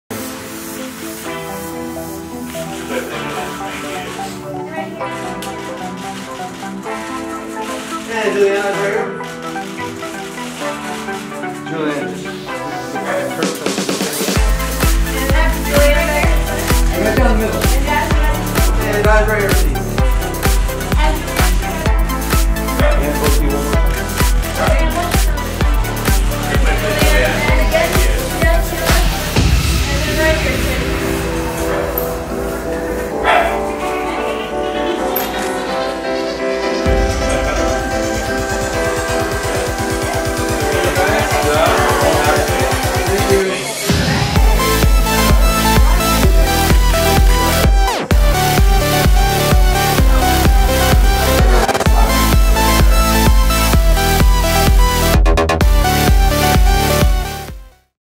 Julianne Hough Celebrates West Coast Debut Of Her New Clothing Collection
LOS ANGELES, CALIFORNIA - APRIL 04: Actress / Dancer Julianne Hough celebrates the West Coast debut of her new clothing collection at Concept 8366 1/2 on April 4, 2016 in Los Angeles, California.